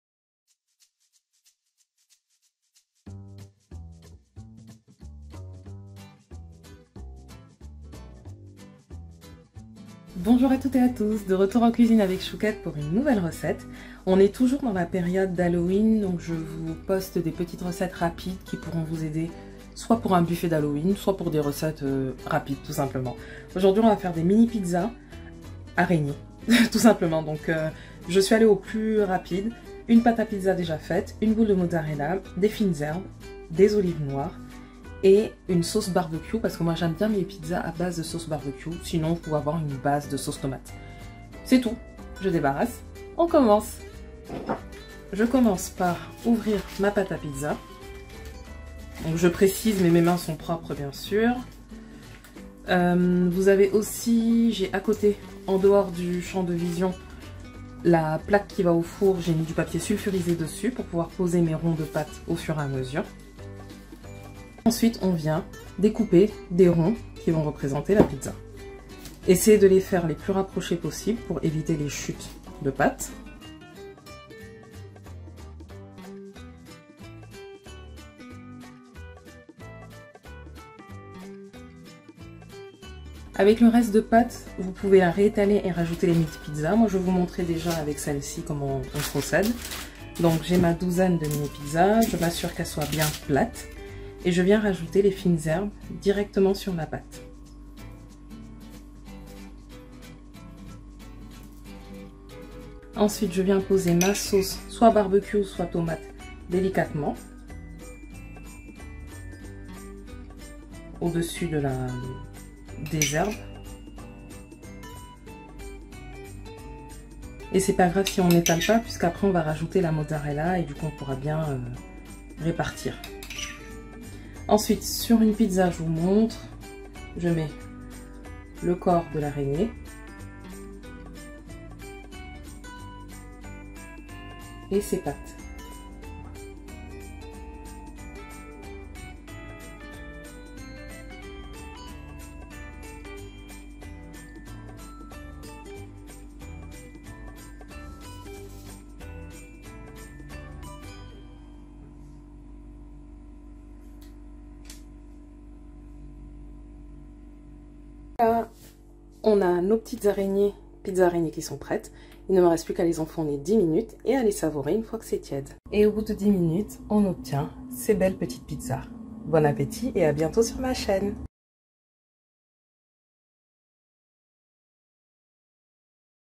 Pizza araignée d'Halloween
Que vous fêtiez Halloween ou non, vous ne pouvez pas passer à côté des décorations et autres recettes associées à cette période de l’année. Cette semaine, je vous propose quelques recettes respectant le cahier des charges suivant : rapide, pas cher, parfait pour un buffet d’Halloween.